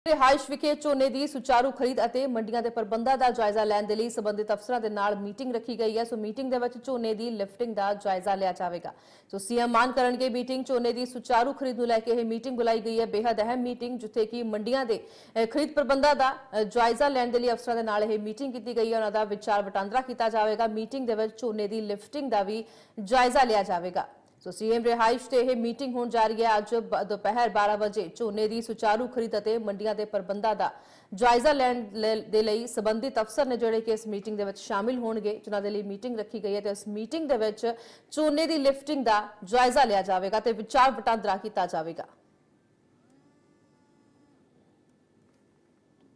CM | Bhagwant Maan | Meeting | Jhona | ਝੋਨੇ ਦੀ ਖਰੀਦ ਨੂੰ ਲੈ ਕੇ ਵੱਡੀ ਮੀਟਿੰਗ || India News Punjab
ਅੱਜ ਮੁੱਖ ਮੰਤਰੀ ਦੀ ਰਿਹਾਇਸ਼ ਵਿਖੇ ਝੋਨੇ ਦੀ ਸੁਚਾਰੂ ਖਰੀਦ ਅਤੇ ਮੰਡੀਆਂ ਦੇ ਪ੍ਰਬੰਧਾਂ ਦਾ ਜਾਇਜ਼ਾ ਲੈਣ ਲਈ ਸੰਬੰਧਿਤ ਅਫ਼ਸਰਾਂ ਨਾਲ ਮੀਟਿੰਗ ਰੱਖੀ ਗਈ ਹੈ। ਮੀਟਿੰਗ ਵਿੱਚ ਝੋਨੇ ਦੀ ਲਿਫਟਿੰਗ ਦਾ ਜਾਇਜ਼ਾ ਲਿਆ ਜਾਵੇਗਾ।

A meeting has been held with the concerned officers today at the Chief Minister's residence to take stock of the smooth procurement of paddy and the arrangements for the mandis. Paddy lifting will be reviewed in the meeting.